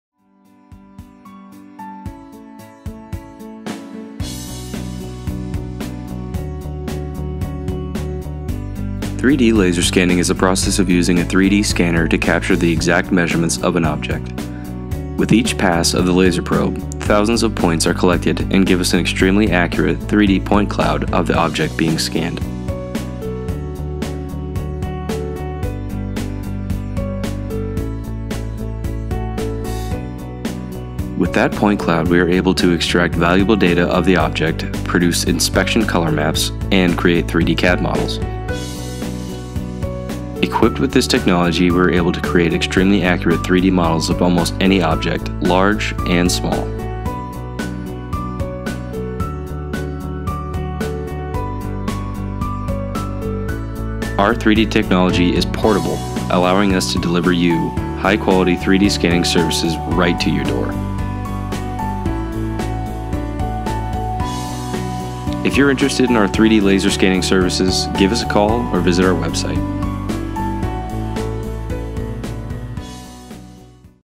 What is 3D Laser Scanning? - Arrival 3D
Learn about 3D laser scanning services from Arrival 3D.